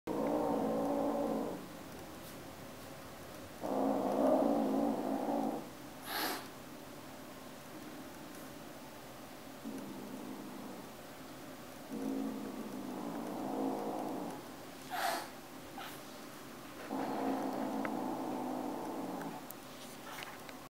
Miepje met haar muis
Miepje Hazelaar van de Moeshoek, 6 weken oud. Laat de anderen het nakijken als het om een muis gaat!